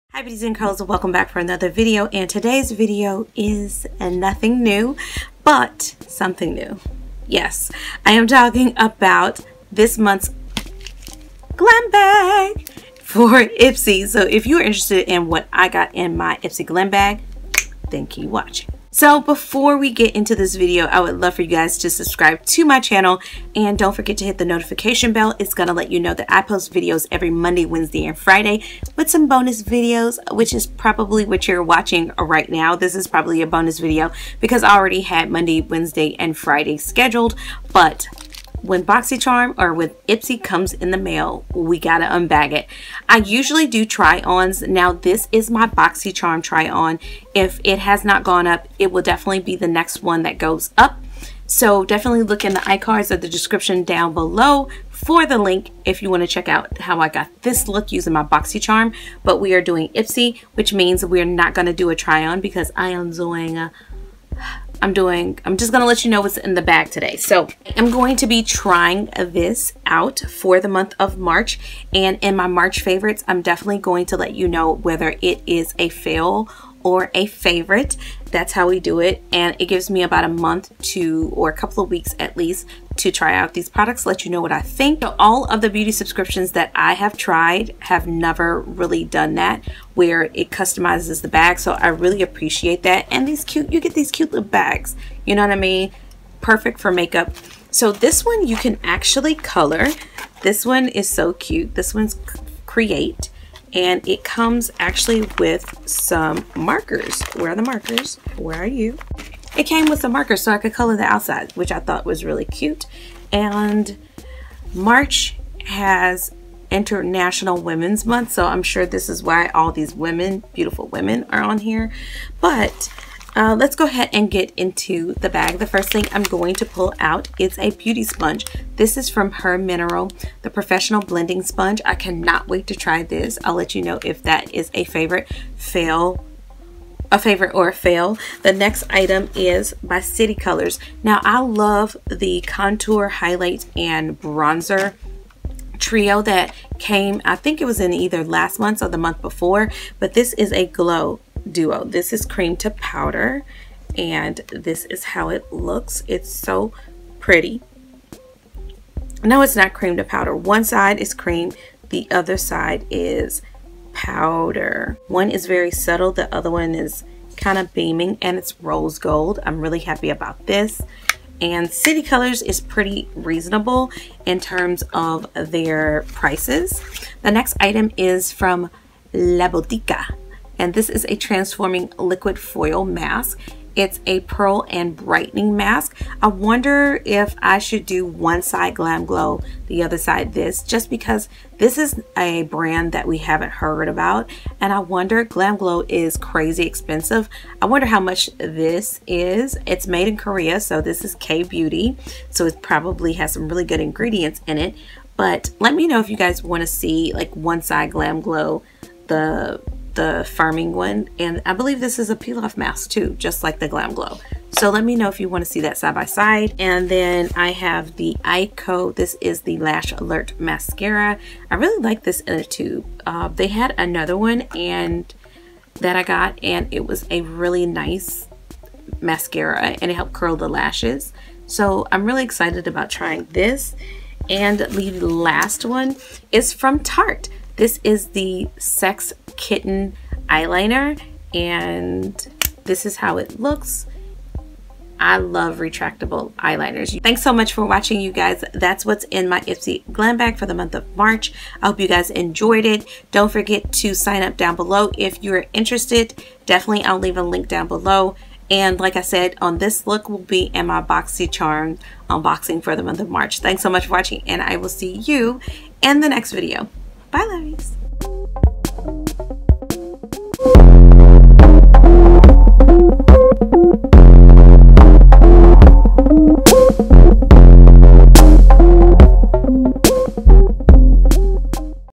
IPSY UNBOXING MARCH 2018 | GIVEAWAY INFO, IS IT WORTH YOUR MONEY? | MelissaQ
HEY DOLL! Is Ipsy Worth The Money??? What do love/regret from the IPSY beauty subscription?

MelissaQ007

✔WRITE ME
Melissa Fykes